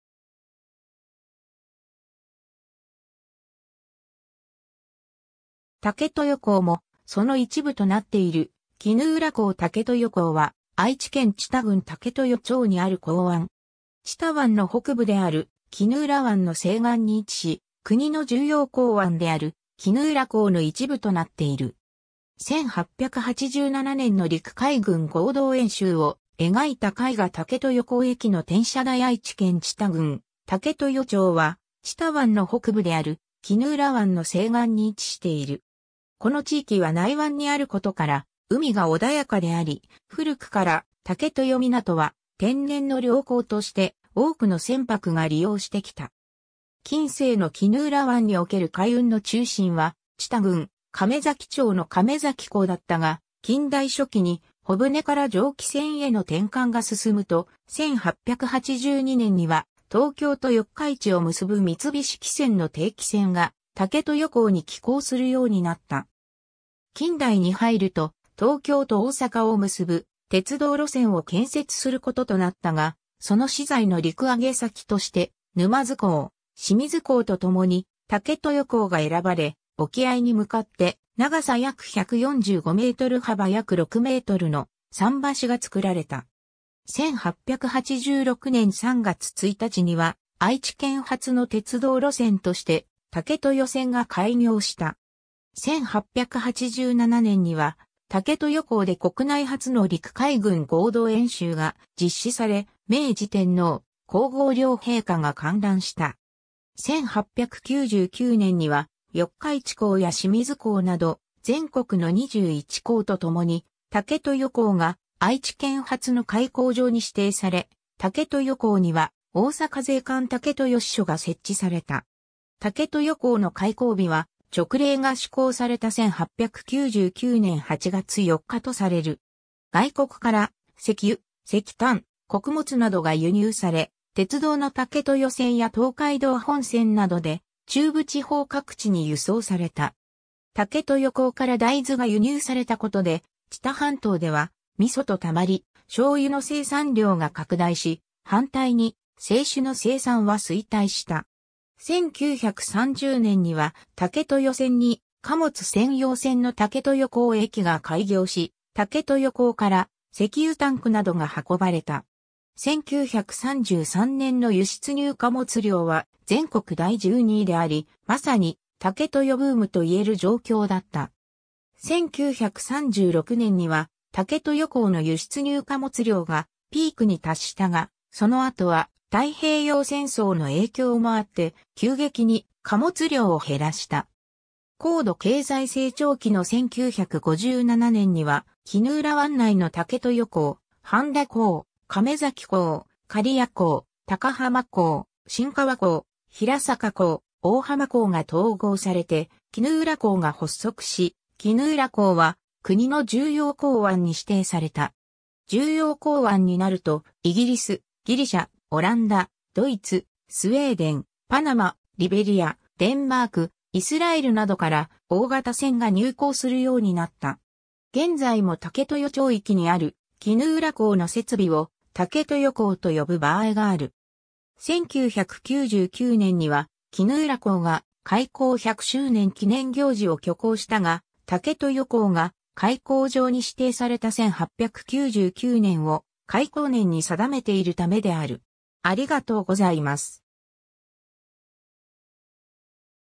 武豊港
武豊港もその一部となっている衣浦港 武豊港（たけとよこう）は、愛知県知多郡武豊町にある港湾。
知多湾の北部である衣浦湾の西岸に位置し、国の重要港湾である衣浦港の一部となっている。
 1887年の陸海軍合同演習を描いた絵画 武豊港駅の転車台 愛知県知多郡武豊町は知多湾の北部である衣浦湾の西岸に位置している。
この地域は内湾にあることから海が穏やかであり、古くから武豊湊（武豊港）は天然の良港として多くの船舶が利用してきた。
近世の衣浦湾における海運の中心は知多郡亀崎町の亀崎港だったが、近代初期に帆船（弁才船）から蒸気船への転換が進むと、1882年（明治15年）には東京と四日市を結ぶ三菱汽船の定期船が武豊港に寄港するようになった。
 近代に入ると東京と大阪を結ぶ鉄道路線を建設することとなったが、その資材の陸揚げ先として沼津港・清水港と共に武豊港が選ばれ、沖合に向かって長さ約145m×幅約6mの桟橋が造られた。
1886年（明治19年）3月1日には愛知県初の鉄道路線として武豊線（現在のJR武豊線）が開業した。
1887年（明治20年）には武豊港で国内初の陸海軍合同演習が実施され、明治天皇・皇后両陛下が観覧した。
 1899年（明治32年）には四日市港や清水港など全国の21港とともに、武豊港が愛知県初の開港場（外国との貿易港）に指定され、武豊港には大阪税関武豊支署が設置された。
武豊港の開港日は勅令が施行された1899年（明治32年）8月4日とされる。
外国から石油・石炭・穀物などが輸入され、鉄道の武豊線や東海道本線などで中部地方各地に輸送された。
武豊港から大豆が輸入されたことで、知多半島では味噌とたまり醤油の生産量が拡大し、反対に清酒の生産は衰退した。
 1930年（昭和5年）には武豊線に貨物専用線の武豊港駅が開業し、武豊港から石油タンクなどが運ばれた。
1933年（昭和8年）の輸出入貨物量は全国第12位であり、まさに武豊ブームと言える状況だった。
1936年（昭和11年）には武豊港の輸出入貨物量がピークに達したが、その後は太平洋戦争の影響もあって急激に貨物量を減らした。
 高度経済成長期の1957年（昭和32年）には、衣浦湾内の武豊港、半田港、亀崎港、刈谷港、高浜港、新川港、平坂港、大浜港が統合されて衣浦港が発足し、衣浦港は国の重要港湾に指定された。
重要港湾になると、イギリス、ギリシャ、オランダ、ドイツ、スウェーデン、パナマ、リベリア、デンマーク、イスラエルなどから大型船が入港するようになった。
現在も武豊町域にある衣浦港の設備を武豊港と呼ぶ場合がある。
1999年（平成11年）には衣浦港が開港百周年記念行事を挙行したが、武豊港が開港場に指定された1899年（明治32年）を開港年に定めているためである。